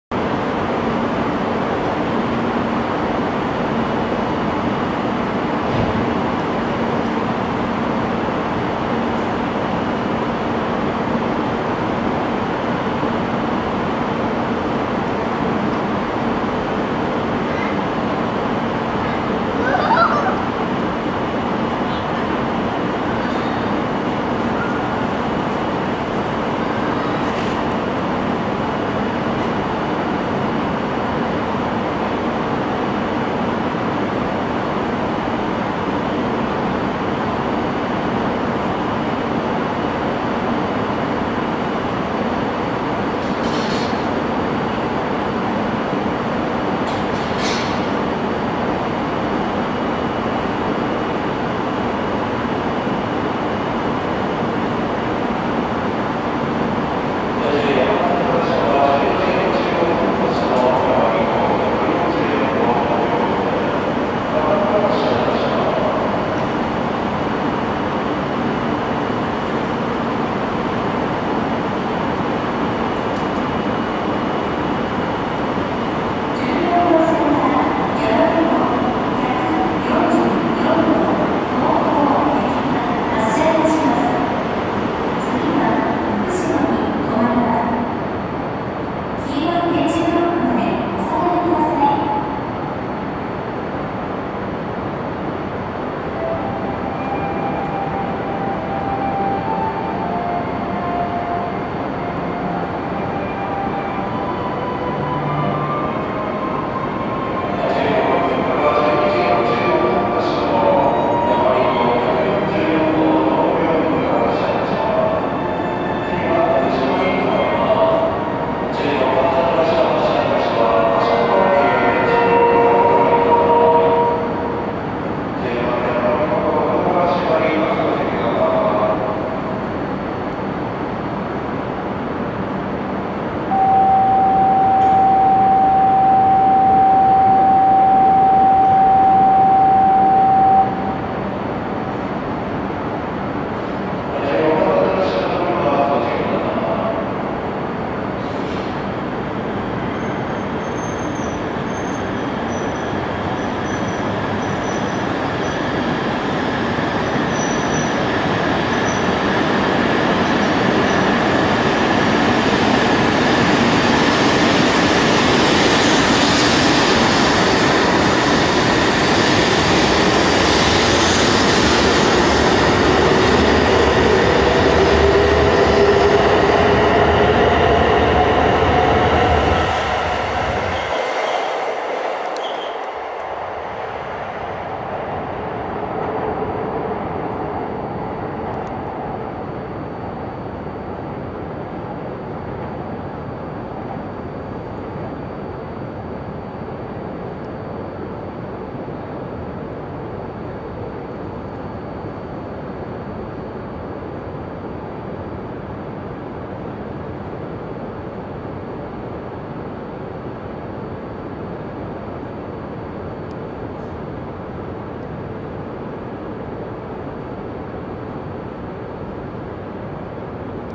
東北新幹線E2系1000番台（フルカラーLED車）やまびこ144号東京行 仙台発車
2024年3月15日（金）撮影
JR東日本新幹線総合車両センター所属E2系1000番台J75編成（後期車）
列車番号144B